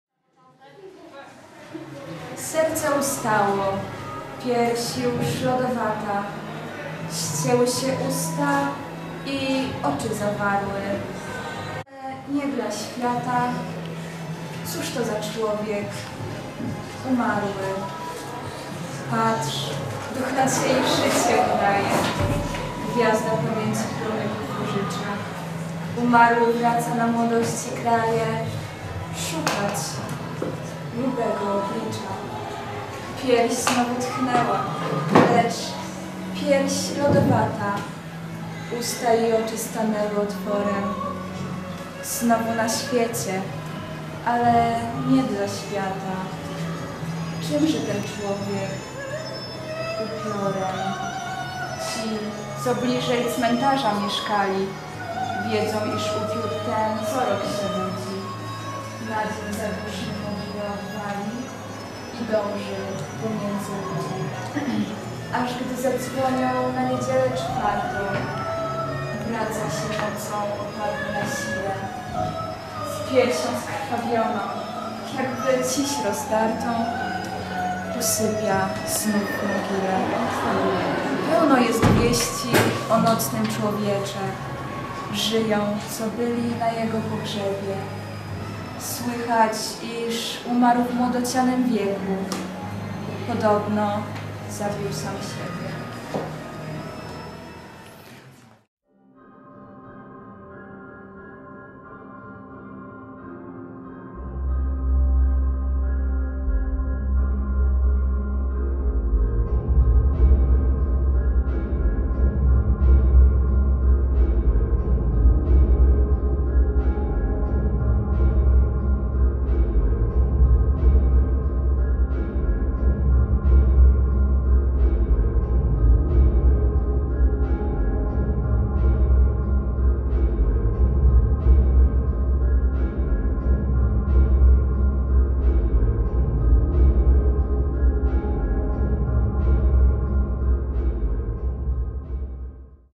Dziady cz. II jako realizacja projektu gimnazjalnego
Klip z finału realizacji obowiązkowego projektu gimnazjalnego